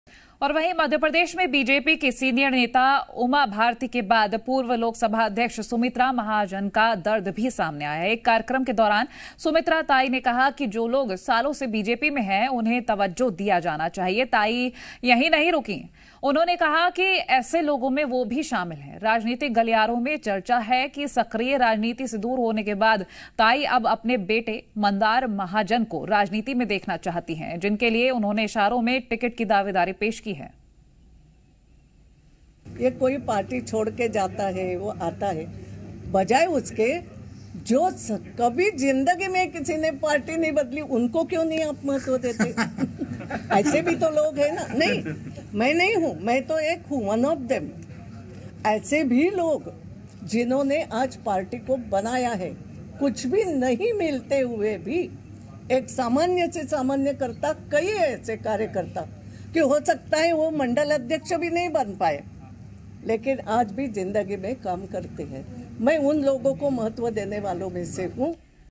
MP News: Uma Bharti के बाद Sumitra Mahajan का छलका दर्द | BJP | Election2023 | ZeeMPCG |Hindi News
MP News: उमा भारती के बाद सुमित्रा महाजन का छलका दर्द, "जो सालों से बीजेपी में है उन्हें तवज्जो देना चाहिए"